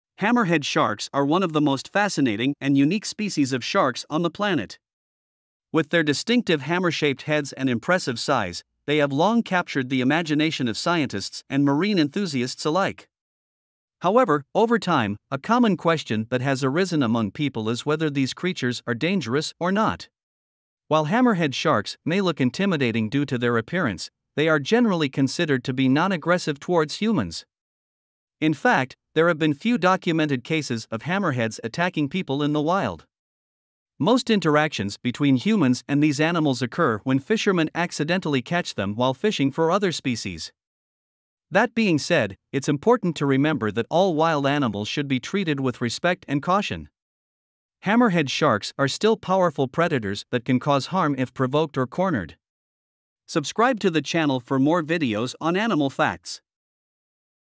Are Hammerhead Sharks Dangerous?
Are Hammerhead Sharks Dangerous? Explore the mystery and intrigue of these misunderstood predators. Learn about their behavior, diet, and how to stay safe in their presence. Get answers to questions such as: Are Hammerhead Sharks a threat to humans? What makes them so unique? Find out what causes Hammerheads to attack, and why they are an essential part of the ocean's ecosystem. Watch this informative video now for all the facts!